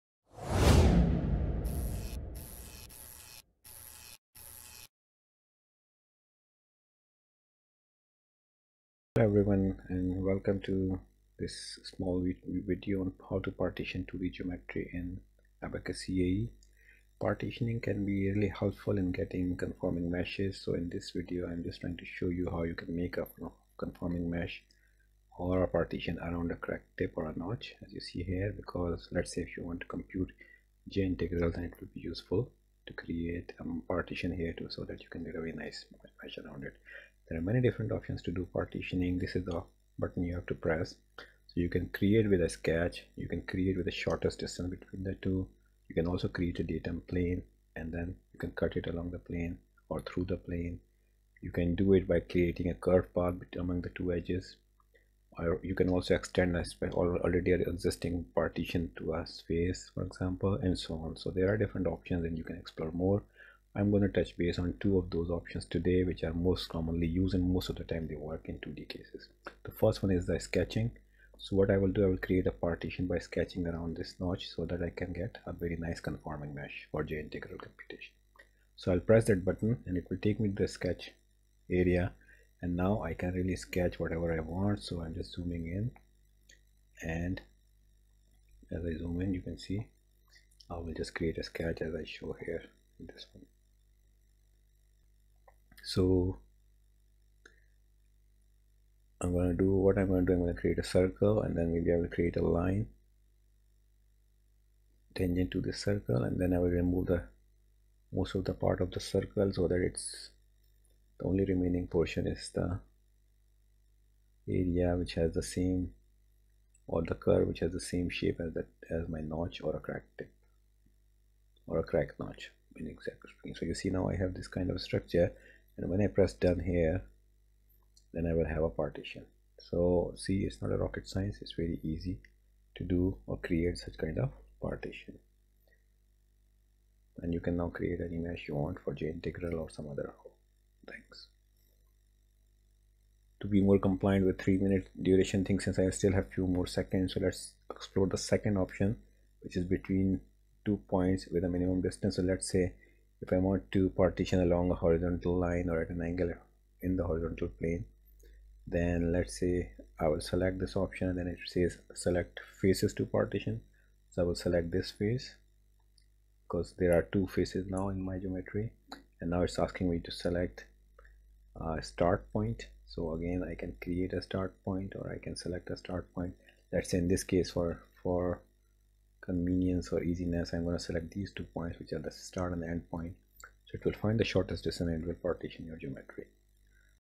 Partition 2D geometry using ABAQUS CAE - ABAQUS Tutorial
This video provides the following in regards to pre-processing in ABAQUS CAE:
- How to create a partition in 2D geometry
- How to create a partition around a crack notch for conforming meshing
- Partitioning using sketch option and using minimum distance between two points in geometry

All proceedings go towards new videos and keeping the webpage going.